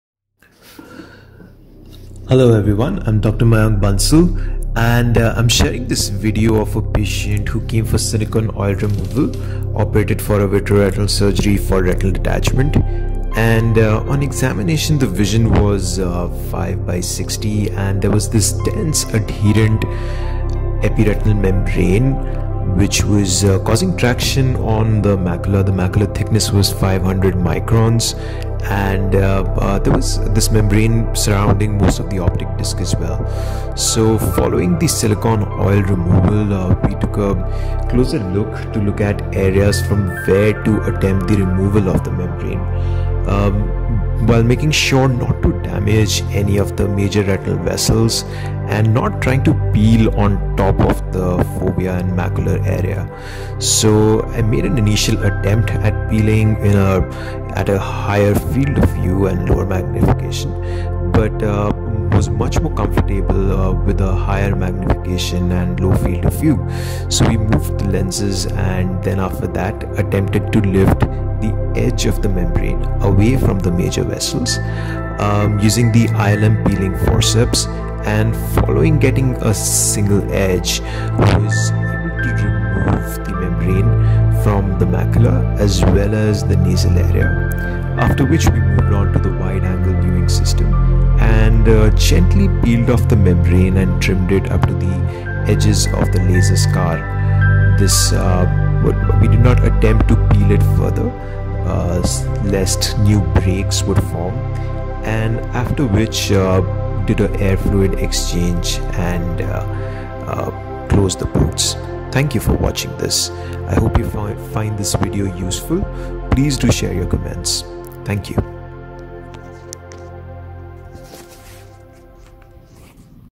Silicon Oil Removal Surgery with Dense Epiretinal Membrane Removal - Dr. Mayank Bansal MD, FRCS
This video shows the silicon oil removal surgery of a patient operated for retinal detachment elsewhere. There was a dense Epiretinal membrane (ERM) - the extent of which suggests retained posterior hyaloid. The emphasis was on initiating the peel away from the macula, so as to not damage that highly sensitive part of the retina. As well as to prevent damage to the major retinal vessels.

Hope you find this video useful, do share your thoughts and comments, they would generate an interesting dialogue.